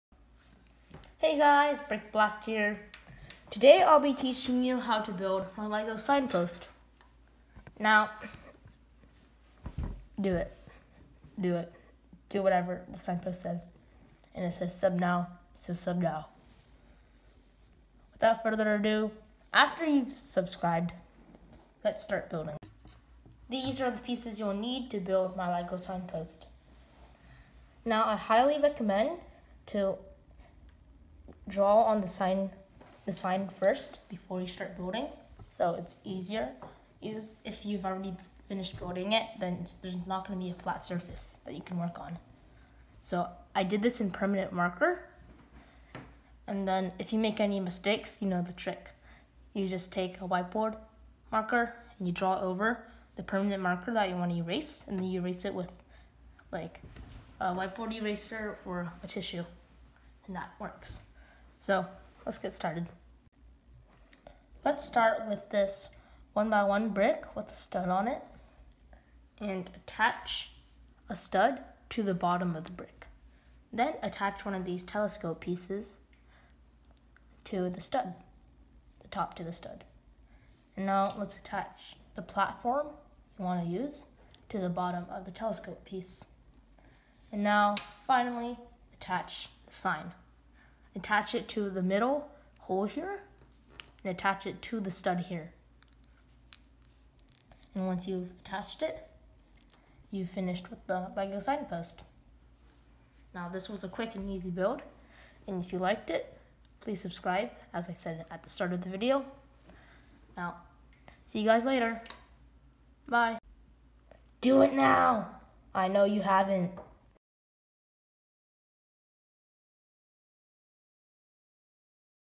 HOW TO Build a Lego Signpost
This is a very simple model, enjoy writing stuff on it.